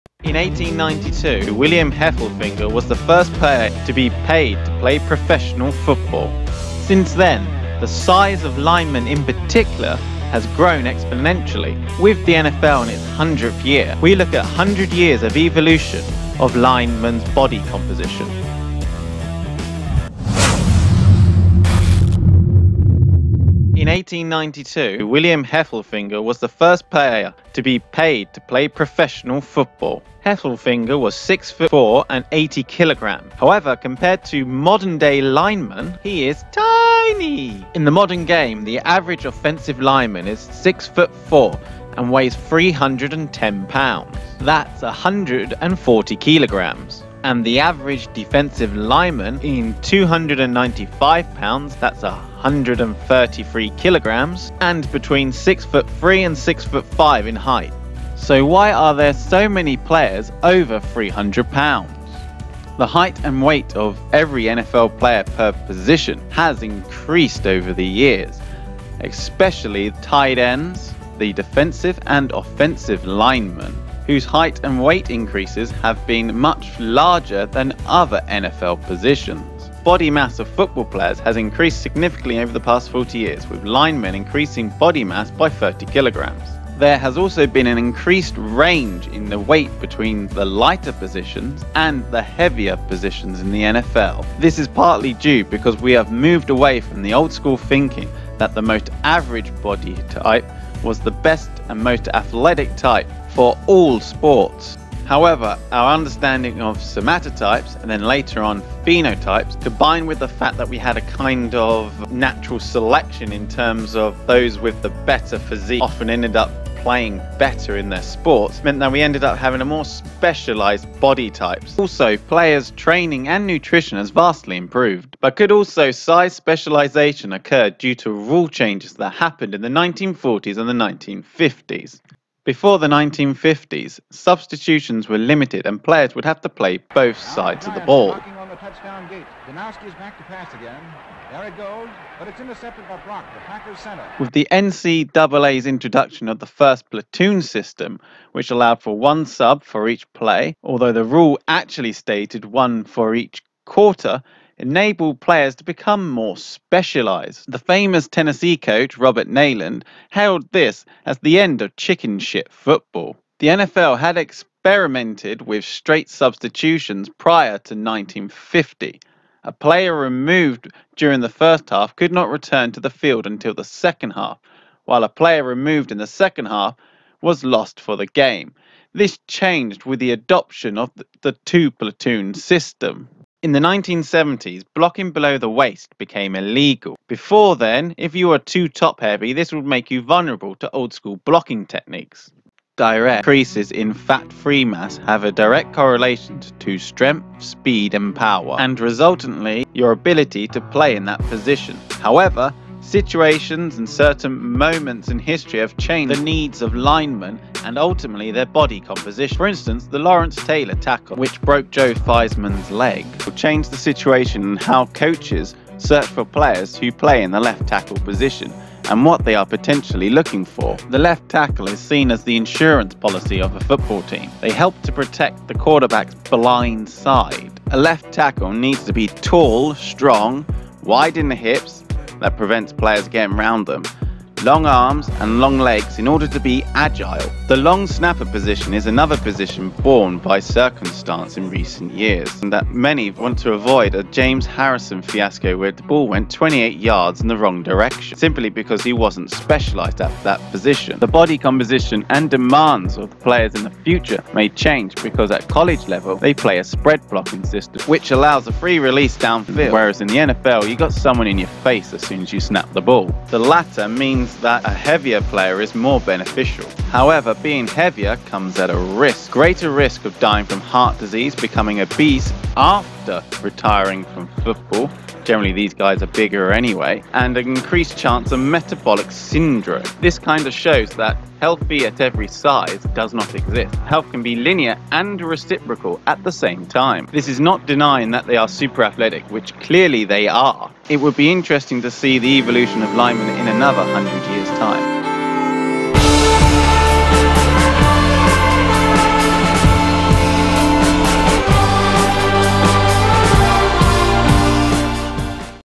NFL 100 Evolution of Linemen| why Linemen are getting Larger
In 1970, only one NFL player weighed over 300 pounds (136kg). That number grew each decade — from 3 in 1980, to 94 in 1990, to 301 in 2000, and finally to 532 in training camps in 2010.

So what changed?

The NFL has changed considerably over the last 100 years. However, the linemen, in particular, have probably changed the most especially after the implementation of the two-platoon system.  They have got not only taller but heavier at a rate that far eclipses other positions.

This video will look at those changes and the sports science behind why that might be, from rule changes to changes in tactics and circumstances.  As variations in different positions.  Likewise, there are positives and negatives as to why to Offensive and Defensive linemen getting larger which will be discussed.

What will NFL Linemen look like in the next 100years?

Refferenes
R.Blender, (Uknown). Average NFL Height and Weight by Position

R. Krishner., (2018). Why NFL teams still can’t live without a long snapper

J H Wilmore & W L Haskell. (1972).  Body composition and endurance capacity of professional football players.

NFL .(2018). Bent but not Broken: History of the NFL

NIOSH. (1994). NFL Mortality Study

A. Marr et al., (2008). Prevalence of Metabolic Syndrome in Retired Football National League Players
note.
* Although at the time of Jack Shapiro's only regular-season appearance for the Staple Island Stapletons vs Minneapolis Red Jackets there were no official specialised positions because of pre-first-platoon NFL rules. He when on the field would have played either in fullback positions or defensive line predominately.